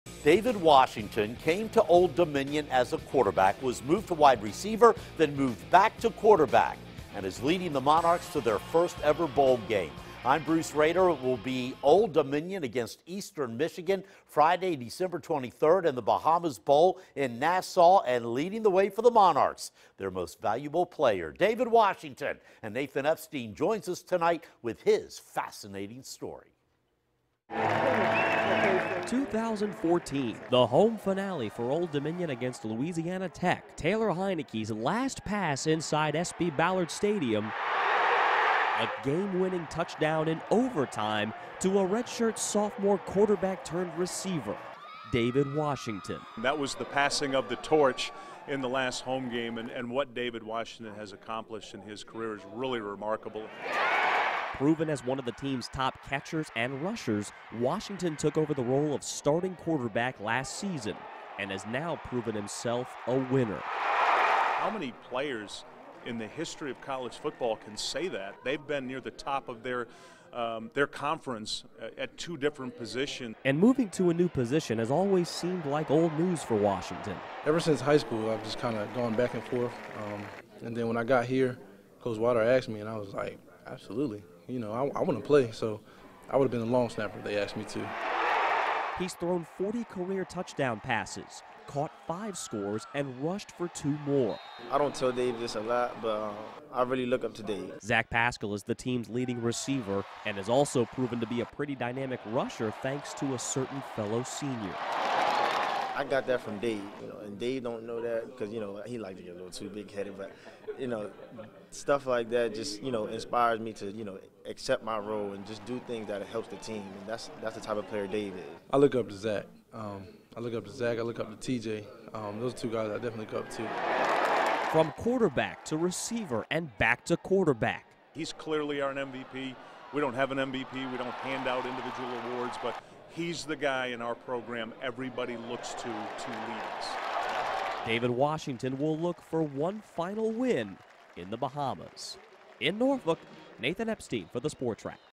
ODU's "MVP" David Washington has thrived
Recruited as a quarterback, David Washington played a bulk of time at receiver before assuming the role as the team's starter behind center. He's proven dominant at both positions since first arriving in Norfolk.